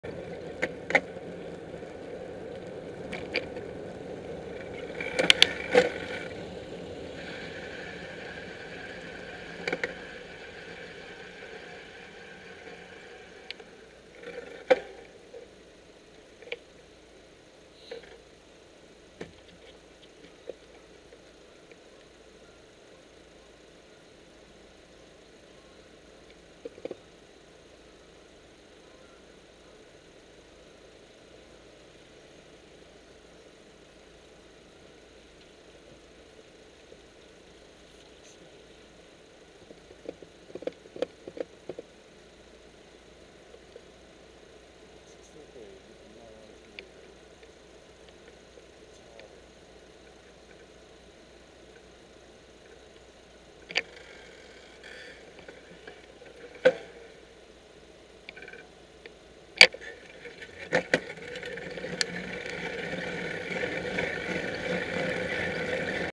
Lights
Situations like this, I can almost understand why some people RLJ. Traffic solid, so no-one was coming through the junction. I could definitely have gone through at no risk to myself or anyone else.

Why can't these lights be on a sensor?